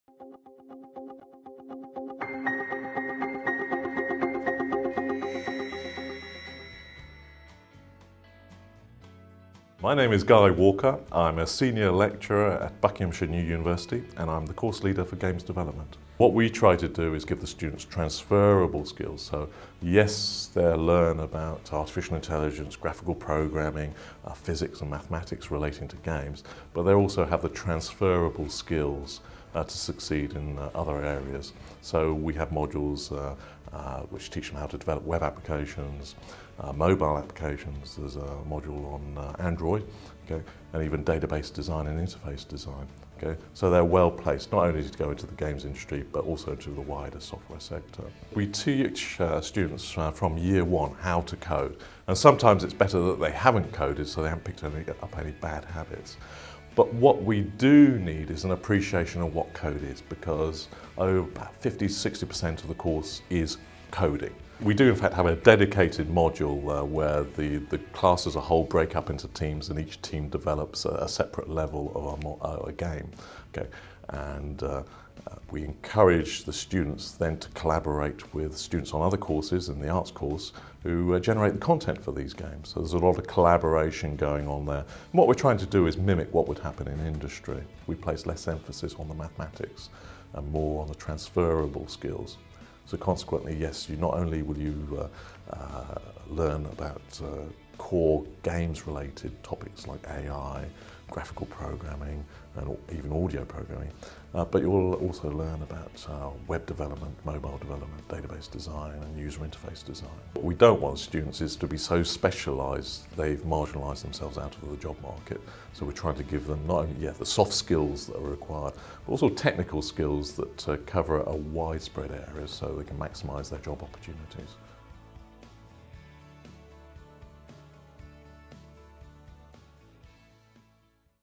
BSc (Hons) Games Development at Bucks New University
There is a continuing demand for skilled developers. This course provides you with a
firm grasp of the modern software engineering techniques used within the
development of computer games.